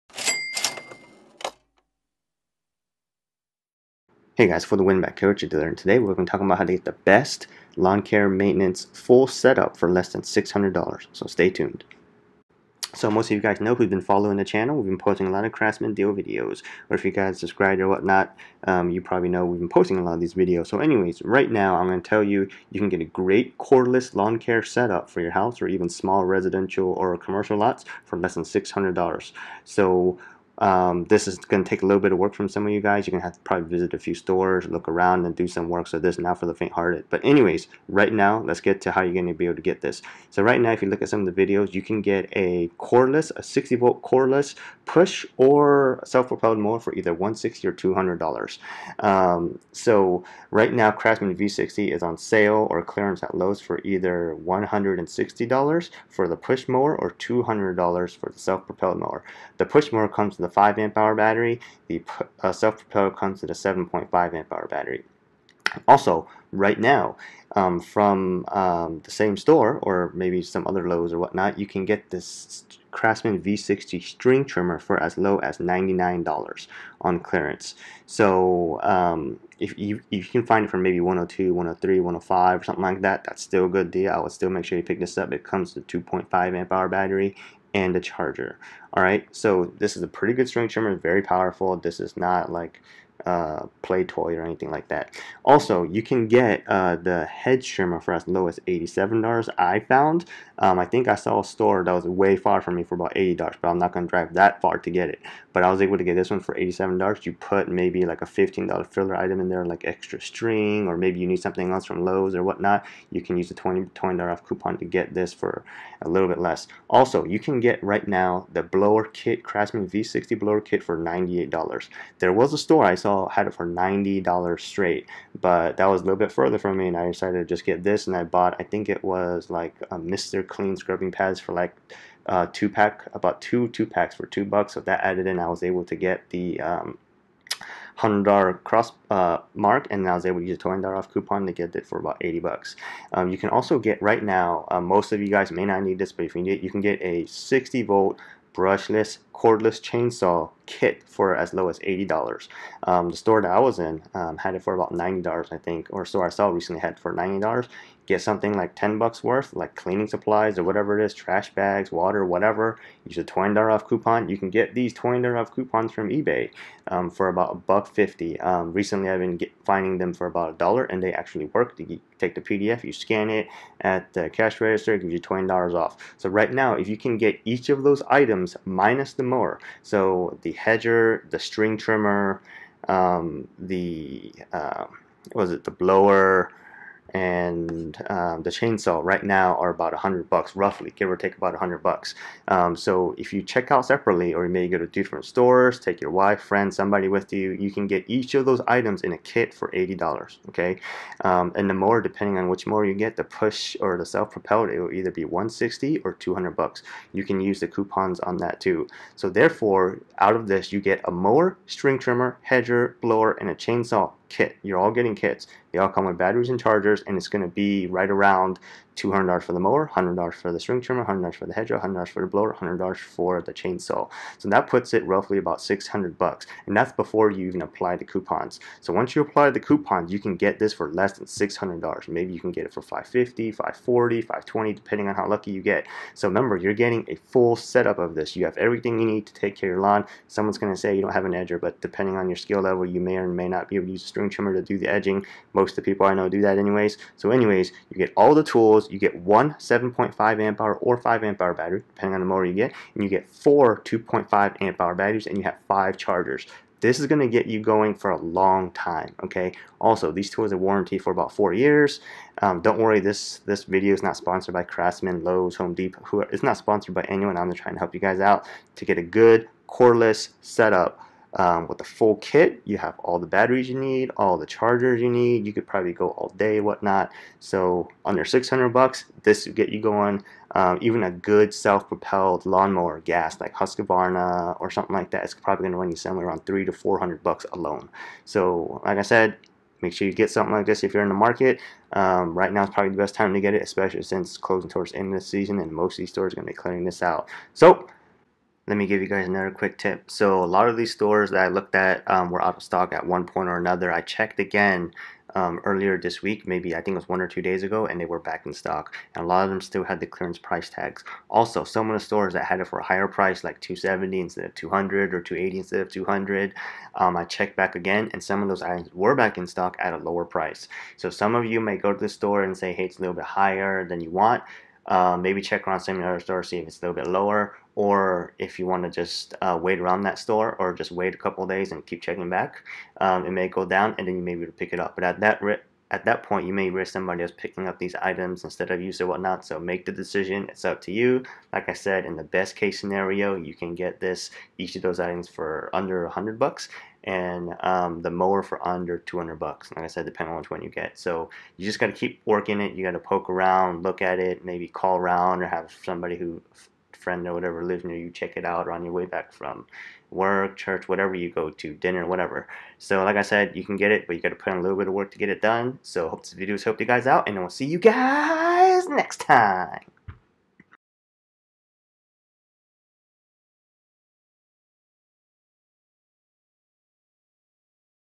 EXPIRED | Deal Alert | How to get a FULL Lawn Care Maintenance KIT setup for less than $600
In this video we go over how to get a full blown lawn care maintenance kit setup for lower than $600. It WILL take some work and time but it can be done, If you get lucky you can get it for even low as $520.

Some of the deals have individual videos on my channel, but thought we'd make a video on a full kit setup.

If you complete the work and get the KIT you will have for $600
1x self propelled / or push mower
1x string trimmer
1x hedger
1x blower
1x chainsaw
4x 2.5ah v60 batteries
1x 7.5ah or 5.0 ah battery
5x chargers

Everything is warrantied for at least 4 years.

These are great cordless tools. Not your cheaper end items. I have personally used all of these tools and can say that I strongly recommend them, especially at this price. At this price, it's a no brainer considering everything you will get. All of the items in the video are KIT items. Meaning each item has the tool, battery, and charger included.

CMCCS660E1
CMCBL760E1
CMCHTS860E1
CMCST960E1

CRAFTSMAN V60 Axial Blower features a 60V brushless motor for powerful performance. The axial blower is fueled by a high performance 2.5 AH lithium ion battery, providing the runtime to complete a 1/4 acre property, and compatibility across other V60 CRAFTSMAN tools. The axial fans design allows for a powerful maximum 600CFM and 110MPH performance. The axial blower features a lightweight design and a variable speed trigger and cruise control for more power or longer runtime. Part of the VERSATRACK™ Wall Organization System. Backed by CRAFTSMAN 4 year limited warranty, 90 day money back guarantee.
Axial Fan Design - Delivers maximum efficiency up to 600CFM and 110MPH
Variable Speed Trigger and Cruise Control - Choose between more power or more runtime
Lightweight Design - Less than 10lbs for decreased user fatigue
Up to 90 Minutes (Low Speed) per charge; Up to 11 Minutes (High Speed) per charge
Includes CMCBL760 axial blower, CMCB6025 V60 MAX 2.5 AH battery, V60 MAX lithium ion charger and an instruction manual

CRAFTSMAN V60 24-in Hedge Trimmer features a 60V motor for powerful performance. The hedge trimmer is fueled by a high capacity 2.5 AH lithium ion battery, and offers compatibility across other V60 CRAFTSMAN tools. The hedge trimmer features a 24-in laser cut, dual action blade, a full bale handle for maximum maneuverability, a 3/4-in blade gap to allow you to quickly cut branches and a Power Saw innovation that allows you to cut branches up to 1-1/2-in thick. Part of the VERSATRACK™ Wall Organization System. Backed by CRAFTSMAN 4 year Limited warranty, 90 day money back guarantee.
24-in Laser Cut Dual Action Blades and Full Bale Handle for Maximum maneuverability
Power Saw Innovation - Cut up to 1-1/2-in thick branches
Up to 3/4-in Cut Capacity - Large blade gap to quickly cut branches
Lightweight - Less than 10lbs with battery
Up to 80 minutes per charge
Includes CMCHT860 24-in hedge trimmer, CMCB6025 V60 MAX 2.5 AH battery, V60 MAX lithium ion charger and an instruction manual

CRAFTSMAN V60 15-in String Trimmer features a high efficiency brushless motor for powerful performance to maximize runtime and motor life. The string trimmer is powered by a high capacity 2.5 AH lithium ion battery, providing the runtime to complete up to a 1/4 acre property, and compatibility across other V60 CRAFTSMAN tools. The string trimmer features a 15-in swath with dual 0.080-in/0.095-in twist line and a bump feed system to help you get the job done quickly, two speed variable switch for higher power or longer runtime, an ergonomic telescoping pole and adjustable auxiliary handle for better ergonomics and control. A Quickwind Spool allows you to replace the line in 3 easy steps, simply align the arrow on the spool with the eyelet, feed the new line through and turn.
Two Speed Variable Switch - For higher power or longer runtime
Quickwind™ Spool System - for ease of line replacement and Bump Feed System - 15-in cutting swath and dual 0.080-in twist line (also accepts 0.095-in)
Ergonomic Telescoping Pole and Adjustable Handle - For better comfort and control
Up to 45 Minutes per charge
Includes CMCST960 V60 15-in string trimmer, CMCB6025 V60 MAX 2.5 AH battery, V60 MAX lithium ion charger, 0.080-in line prewound in spool, 0.095-in line sample, auxiliary handle and guard